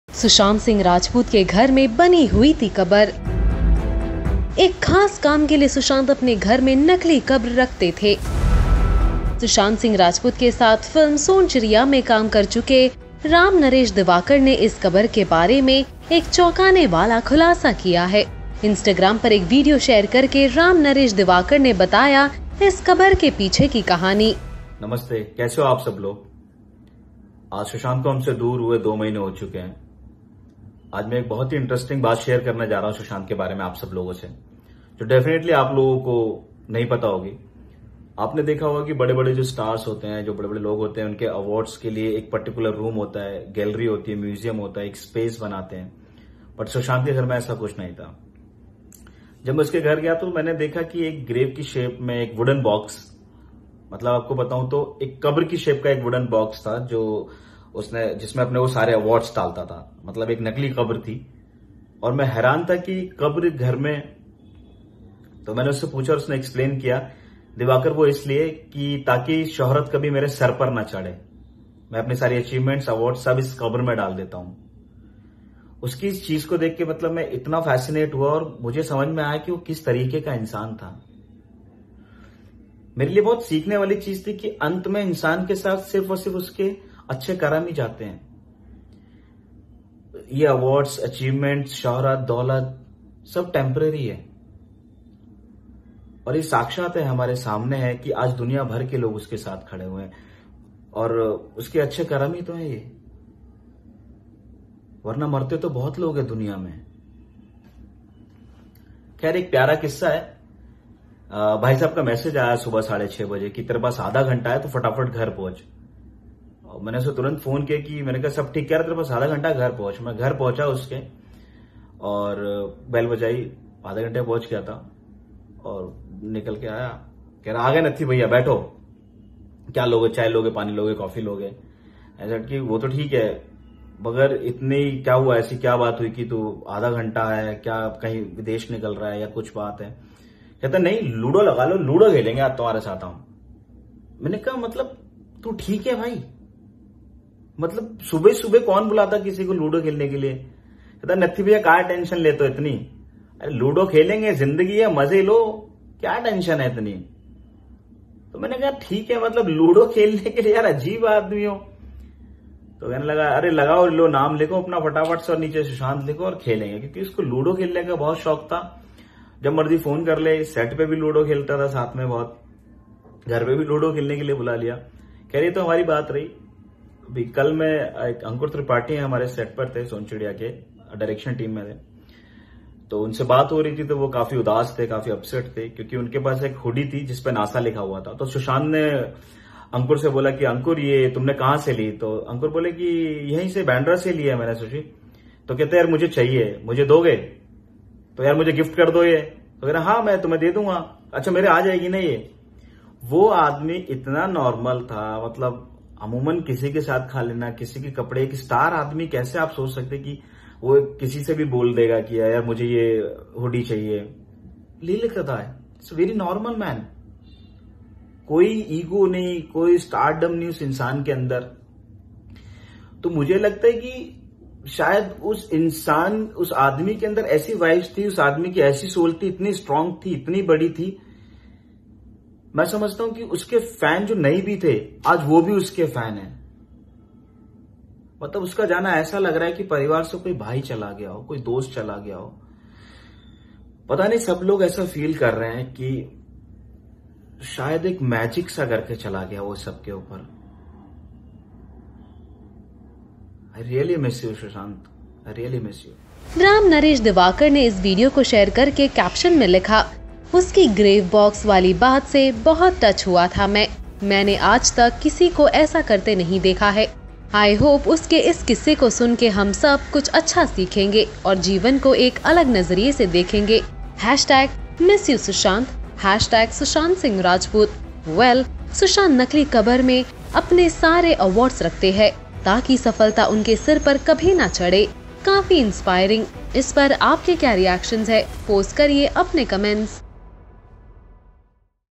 Sushant Singh Rajput's GRAVE BOX In The House | Sonchiriya's Co-Star Shocking Revelation
Did you know Late Actor Sushant Singh Rajput used to keep Graves in his house? His co-star and friend Ram Naresh Diwakar reveals the mystery behind the grave box. Watch the video.